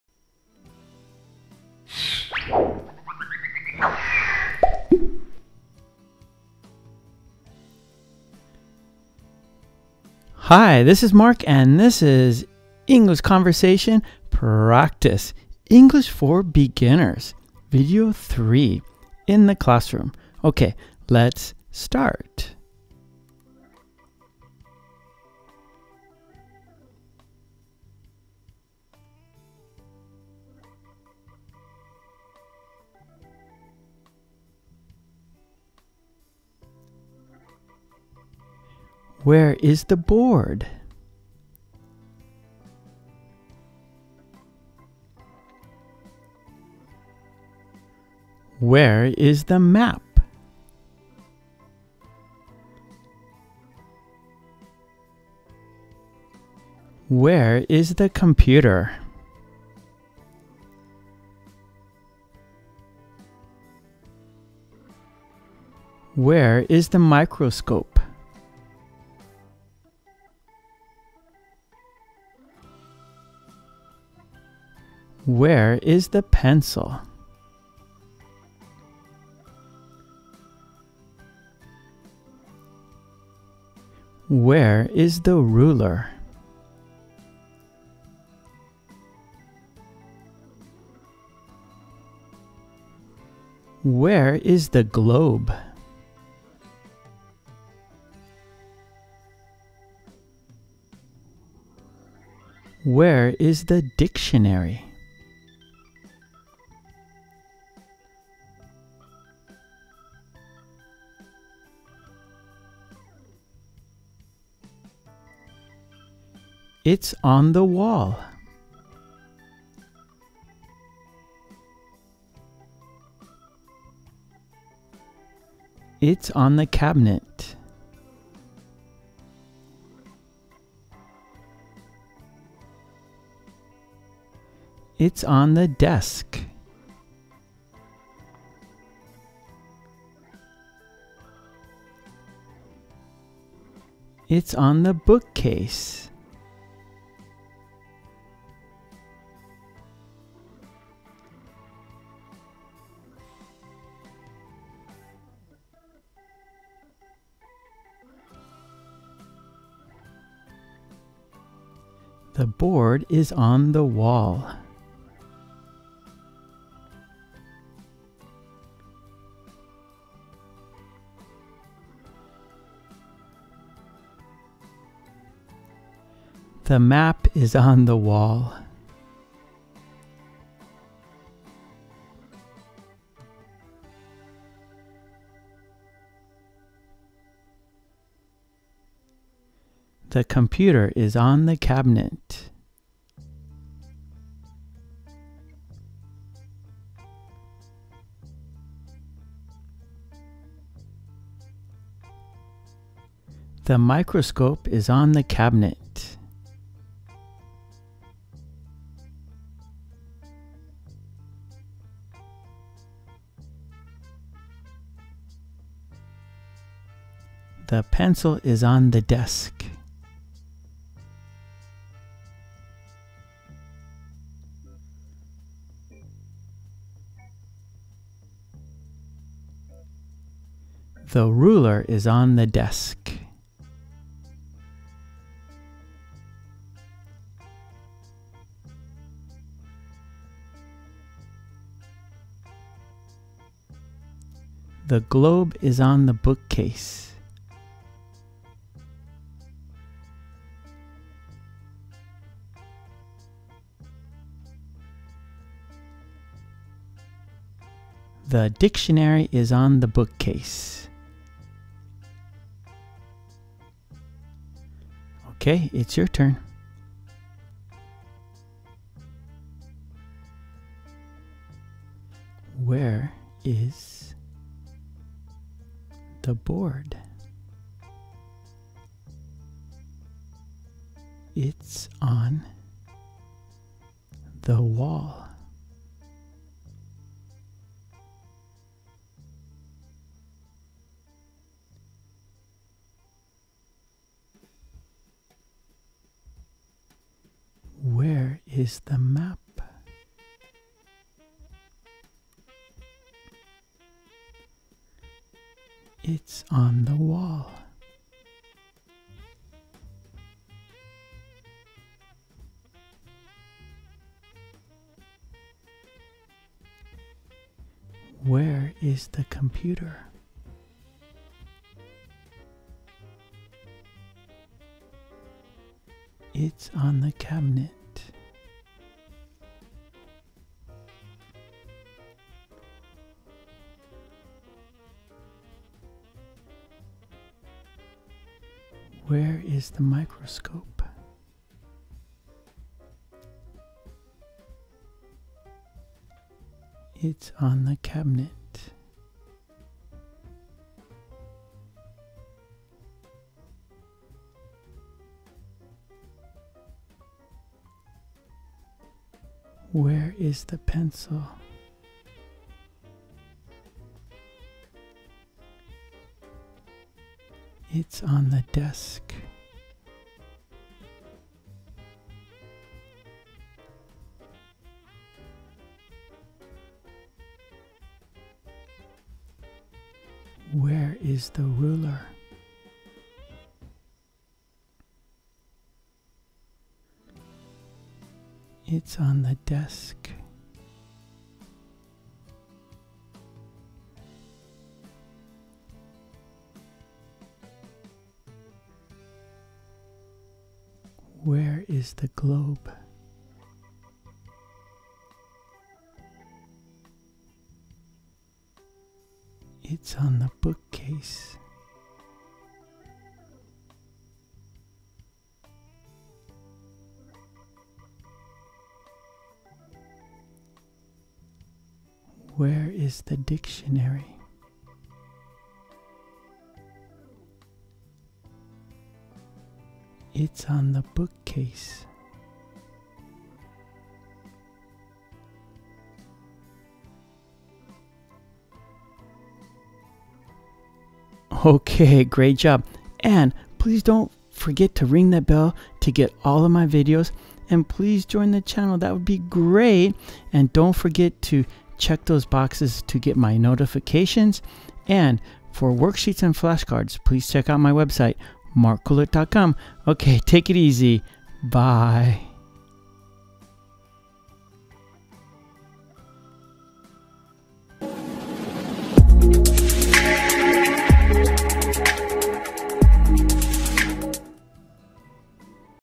English For Beginners Series - the classroom (#3) | Mark Kulek - ESL
In The Classroom - English for Beginners. Learn to ask where something is, learn to answer where something is by using on and make sentences in English. You will learn these school room items: a board, a map, a wall, a cabinet, a microscope, a computer, a desk, a pencil, a ruler, a bookcase, a globe, a dictionary. Each conversation is illustrated and color coded for better understanding. This is an interactive listening and speaking video. Please listen and then repeat after Mark. Have fun learning English and speak English now.

⏱TimeStamps⏱
0:00 Intro
0:30 Where is the ... ? (classroom items)
1:49 It's on the ... (wall, cabinet, desk, bookcase)
2:30 Sentences
4:26 Do It Yourself (on your own)

English in use:
Where is the board? It's on the wall.
Where is the map? It's on the wall.
Where is the computer? It's on the cabinet.
Where is the microscope? It's on the cabinet.
Where is the pencil? It's on the desk.
Where is the ruler? It's on the desk.
Where is the globe? It's on the bookcase.
Where is the dictionary? It's on the bookcase.

📕 Mark's book: 50 Very Short Conversations